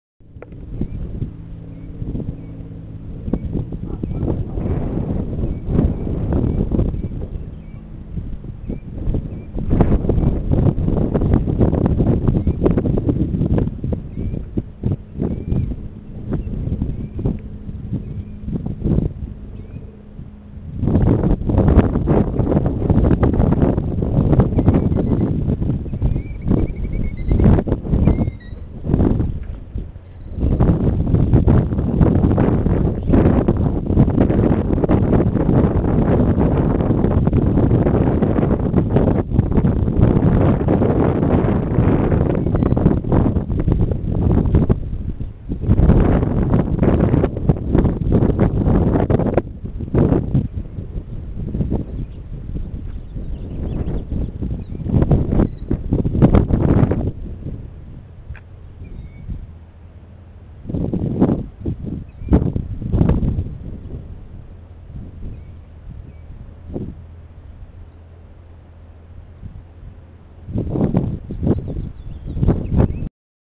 Three Killdeer Chicks
Three killdeer chicks rounded up by adult.